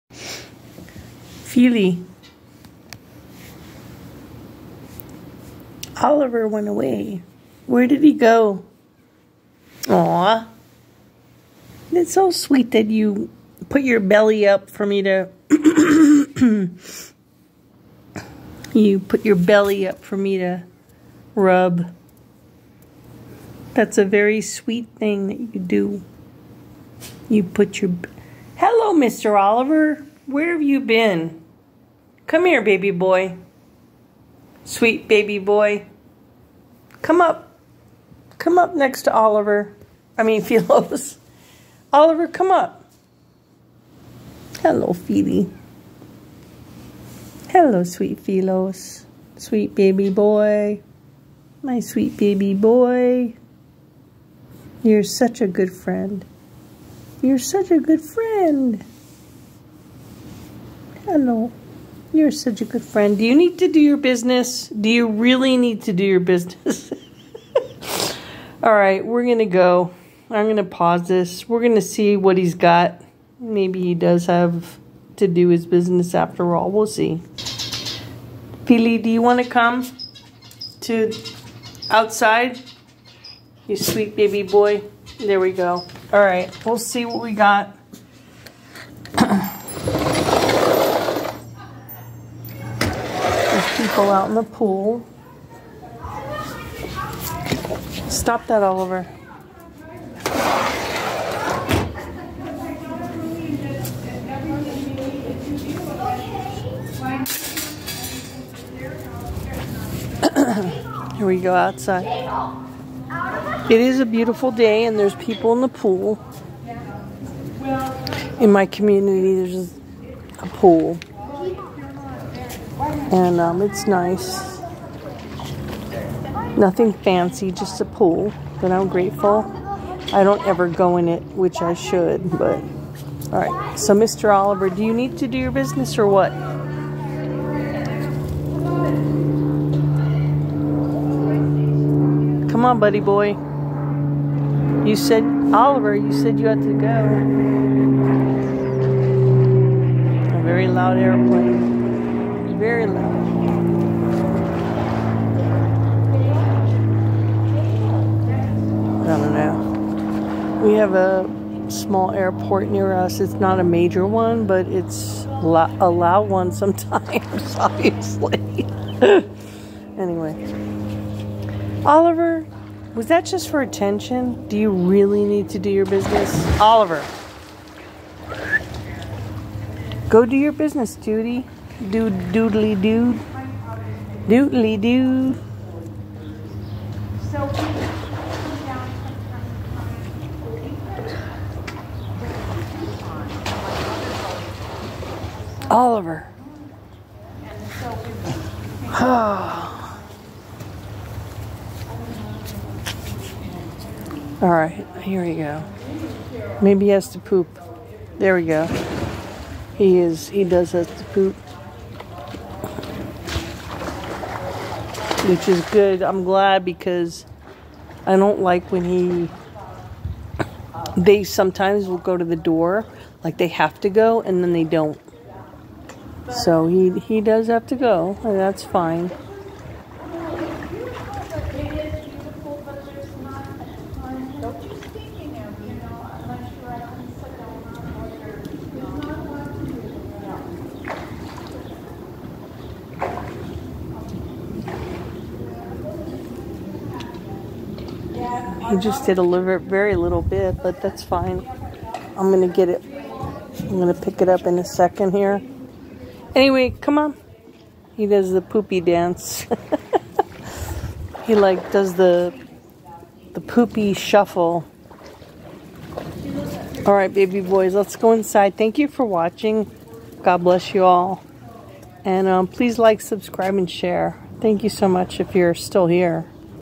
PLEASE DO YOUR BUSINESS DUDELY DUDE!!🐶💛🐶🤗🌼
Sometimes I'm not sure they need to do their business,  or if they just want to go out!!!

Turns out Oliver does need to do his business!